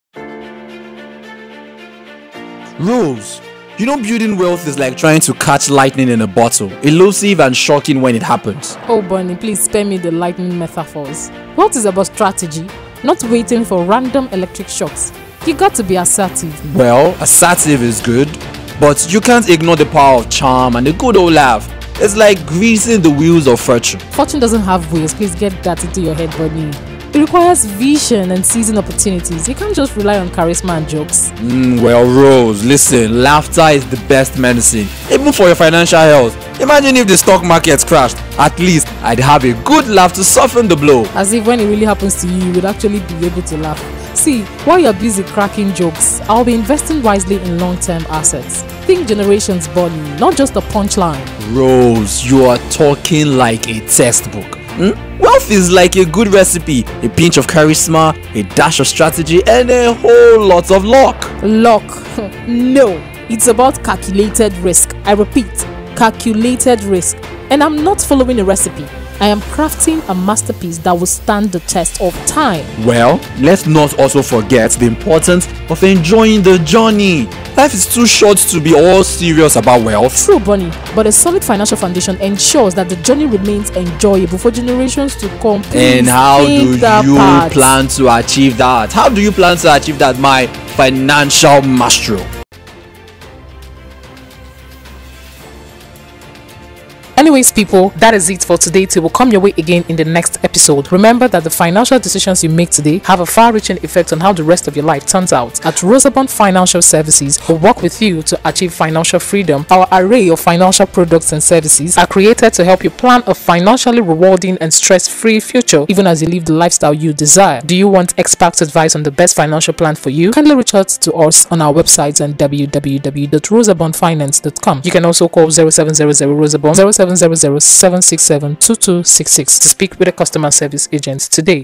Unlock Generational Wealth: Rose & Bonnie's Golden Secrets Revealed! 💰✨
🌟 Dive into the treasure trove of Generational Wealth with Rose & Bonnie! In this glittering episode, discover the secret sauce to creating a lasting financial legacy. 🗝️💎

Join our dynamic duo as they explore captivating stories, powerful strategies, and surprising tips that will reshape your understanding of wealth. Get ready for a rollercoaster of insights, laughter, and eye-opening moments that will ignite your journey to building a family fortune!

🎙️ Plug in your headphones and embark on this transformative conversation that's turning dreams into reality. Listen now on Spotify It's not just another podcast – it's your roadmap to financial dreams!

Don't miss out on this golden ticket to a wealthier, wiser tomorrow! 🚀💡